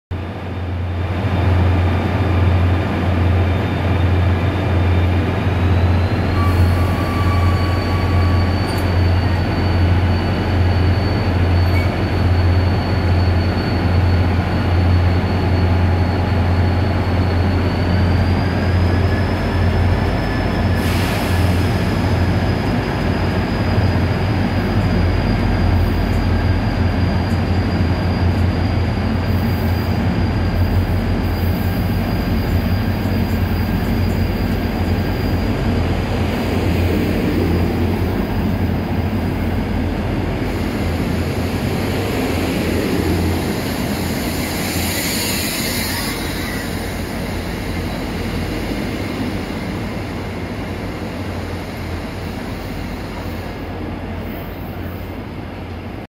용산행 무궁화호1560열차 수원역 출발
2023/9/9 광천역-수원역까지 이용한 무궁화호
1560열차